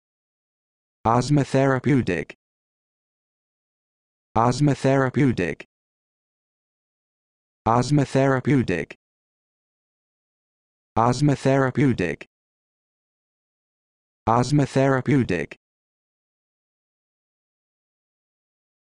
How to Pronounce Osmotherapeutic
This video shows you how to pronounce Osmotherapeutic